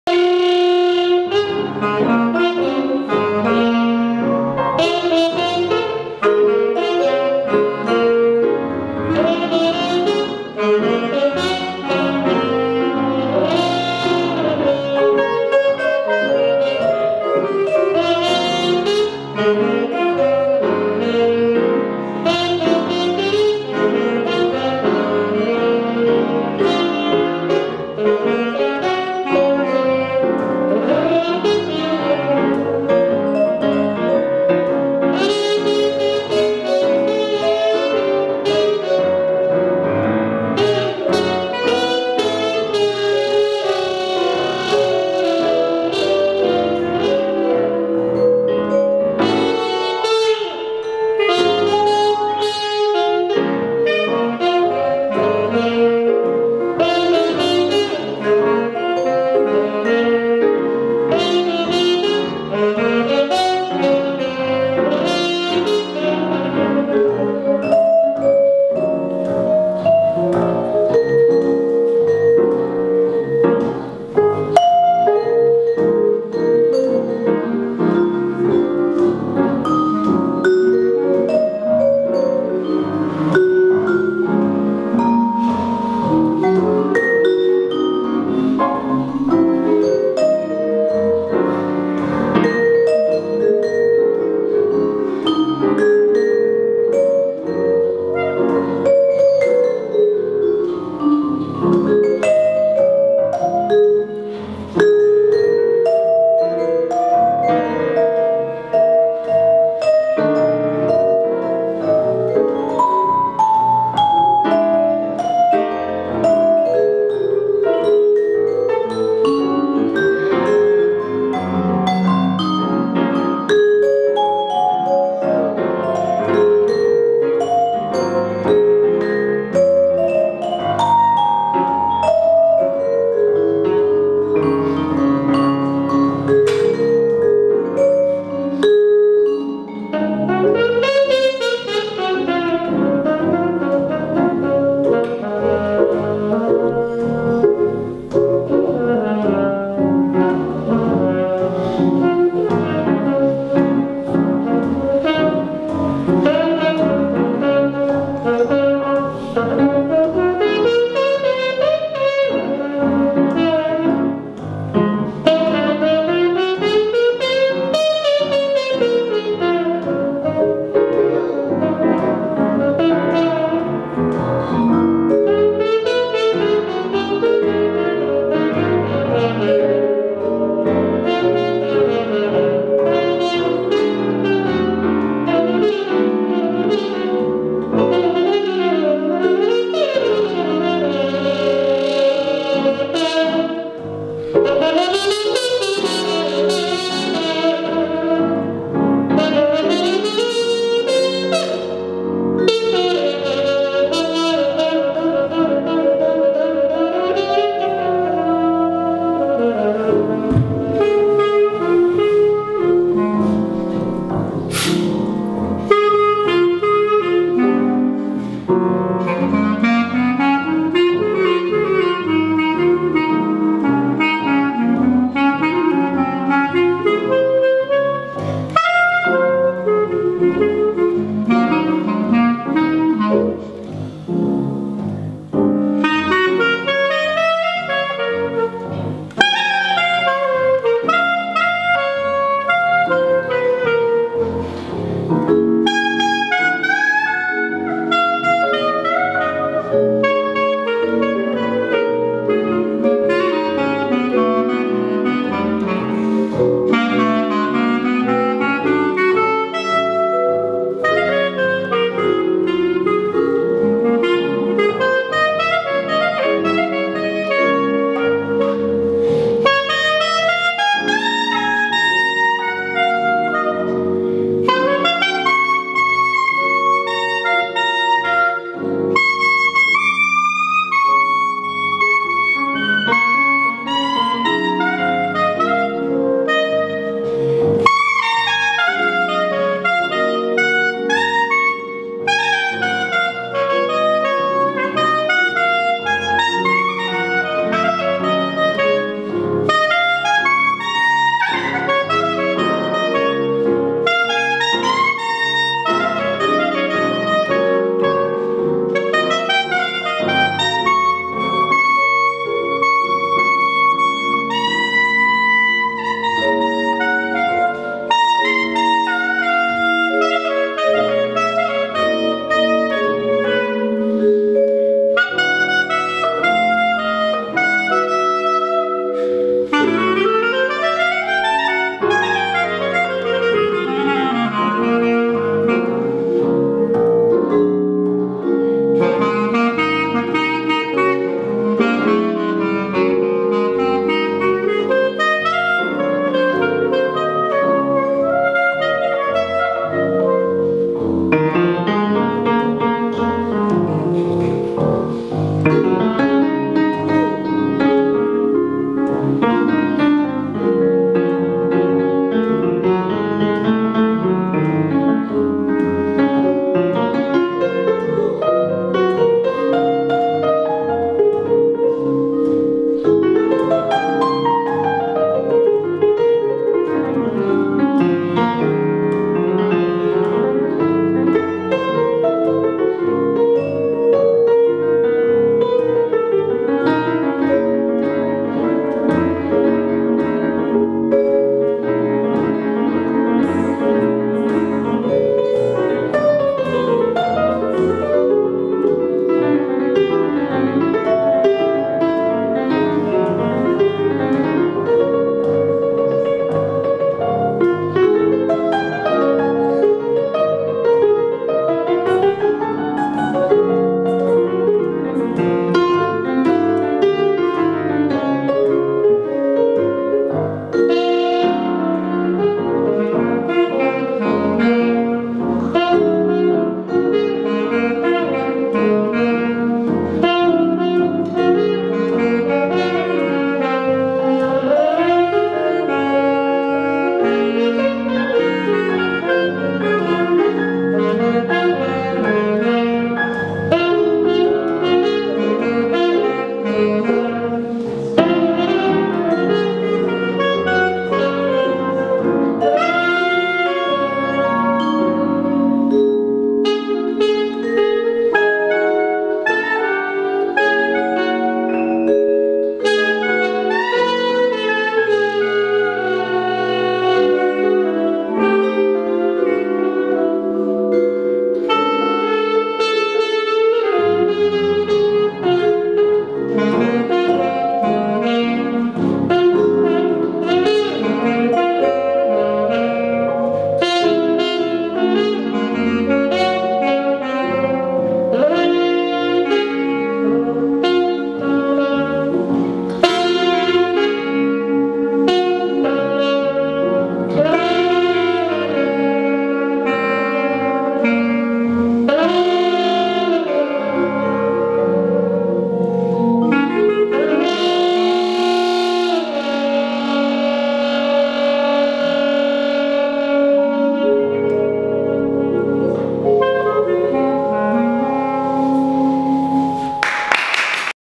L'ISLE SAINT-LOUIS EXTRACT CONCERT QUARTET ANDREUCCI - CANTINI - SCHINA - DA PREDA 6
ROBERTO ANDREUCCI PIANOFORTE
RENATO CANTINI TROMBA
FRANCESCO SCHINA SAX TENORE
IAN DA PREDA VIBRAFONO
29/10/2015 HD L'ISLE SAINT-LOUIS FLORENCE